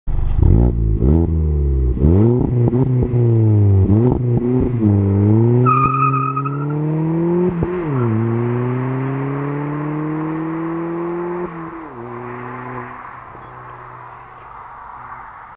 Polo 1.2 TSI R-line with Friedrich Motorsport exhaust
Polo 1.2 TSI R-line with Friedrich motorsport exhaust, removed middle resonator & sport catalyst